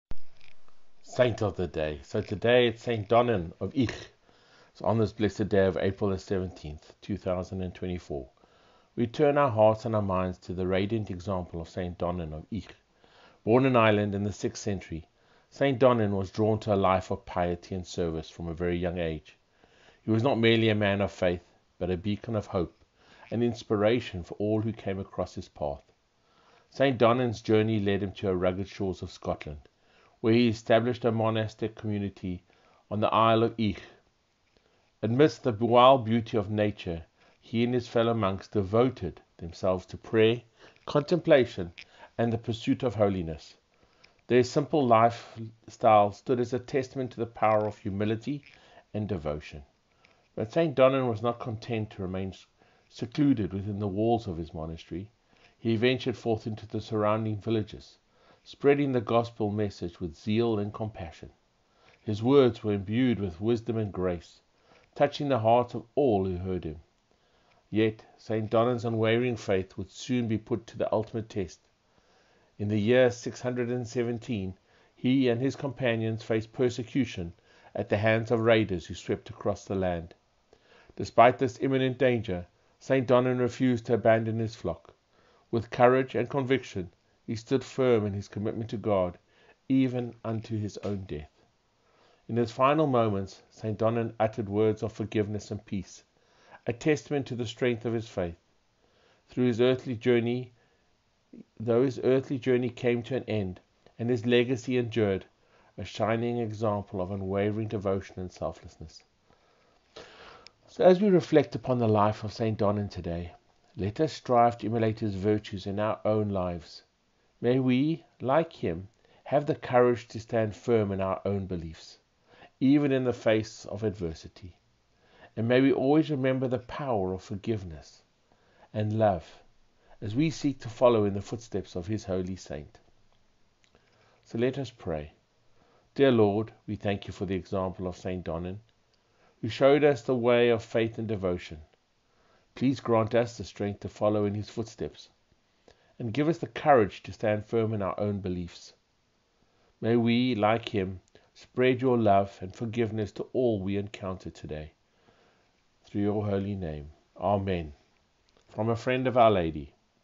Saint Donnan of Eigg - 17 April 2024
Saint Donnan of Eigg -
On this blessed day of April 17th 2024, let us turn our hearts and minds to the radiant example of Saint Donnan of Eigg. Born in Ireland in the sixth century, Saint Donnan was drawn to a life of piety and service from a young age. He was not merely a man of faith, but a beacon of hope and inspiration for all who crossed his path.
Saint Donnan's journey led him to the rugged shores of Scotland, where he established a monastic community on the Isle of Eigg. Amidst the wild beauty of nature, he and his fellow monks devoted themselves to prayer, contemplation, and the pursuit of holiness. Their simple lifestyle stood as a testament to the power of humility and devotion.
But Saint Donnan was not content to remain secluded within the walls of his monastery. He ventured forth into the surrounding villages, spreading the Gospel message with zeal and compassion. His words were imbued with wisdom and grace, touching the hearts of all who heard them.
Yet, Saint Donnan's unwavering faith would soon be put to the ultimate test. In the year 617, he and his companions faced persecution at the hands of raiders who swept across the land. Despite the imminent danger, Saint Donnan refused to abandon his flock. With courage and conviction, he stood firm in his commitment to God, even unto death.
In his final moments, Saint Donnan uttered words of forgiveness and peace, a testament to the strength of his faith. Though his earthly journey came to an end, his legacy endured, a shining example of unwavering devotion and selflessness.
As we reflect upon the life of Saint Donnan, let us strive to emulate his virtues in our own lives. May we, like him, have the courage to stand firm in our beliefs, even in the face of adversity. And may we always remember the power of forgiveness and love, as we seek to follow in the footsteps of this holy saint.
Let us pray:
Dear Lord, we thank you for the example of Saint Donnan, who showed us the way of faith and devotion. Grant us the strength to follow in his footsteps, and the courage to stand firm in our beliefs. May we, like him, spread your love and forgiveness to all we encounter. Amen.
A Friend Of OUR LADY